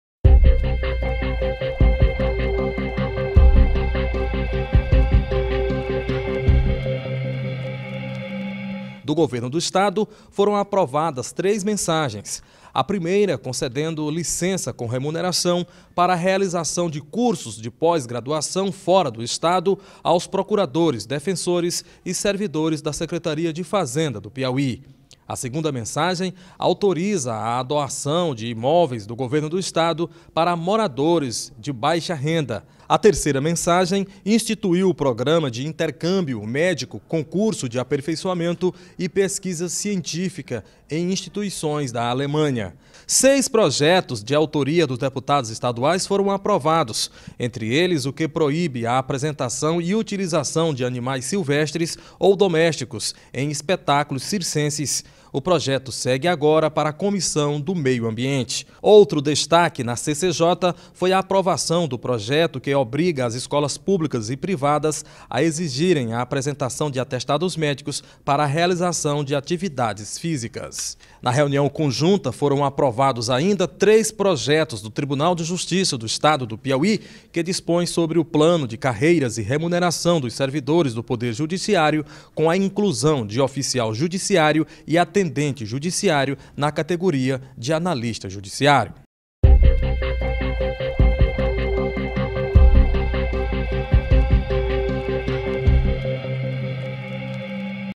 Reunião conjunta da Alepi aprova vários projetos
Reunião conjunta das comissões técnicas da Assembleia Legislativa do Piauí consegue aprovar vários projetos tanto da Casa como do Executivo e do Judiciário. Confira a matéria com Bartolomeu Almeida.